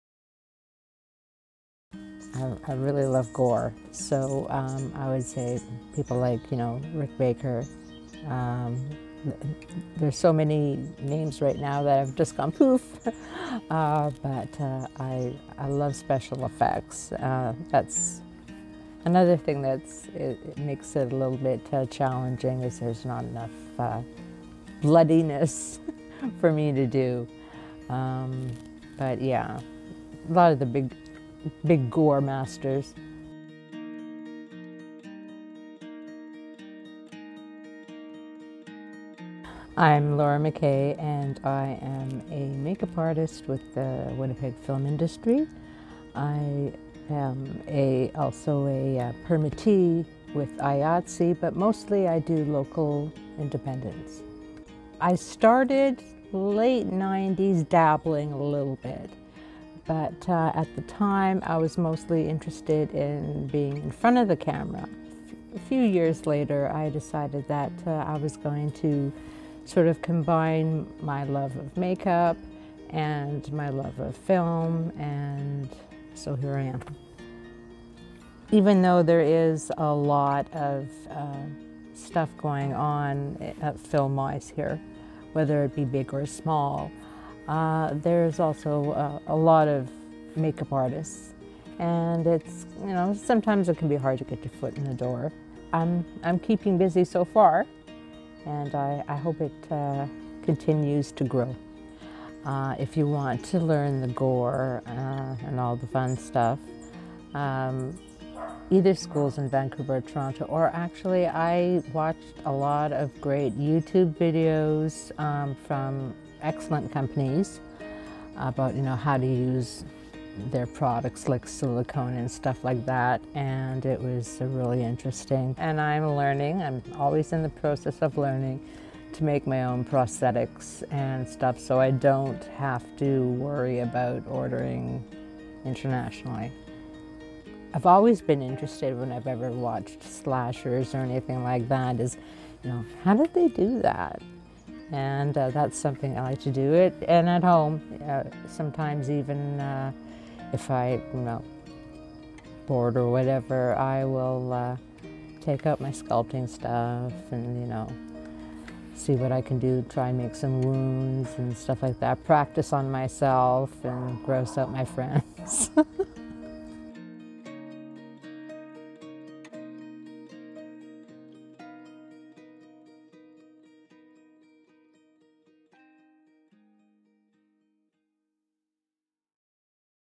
Laura Mackay | Winnipeg Make-Up Artist | Peg Talent Docuseries
A feature on Laura Mackay a Winnipeg Make-Up Artist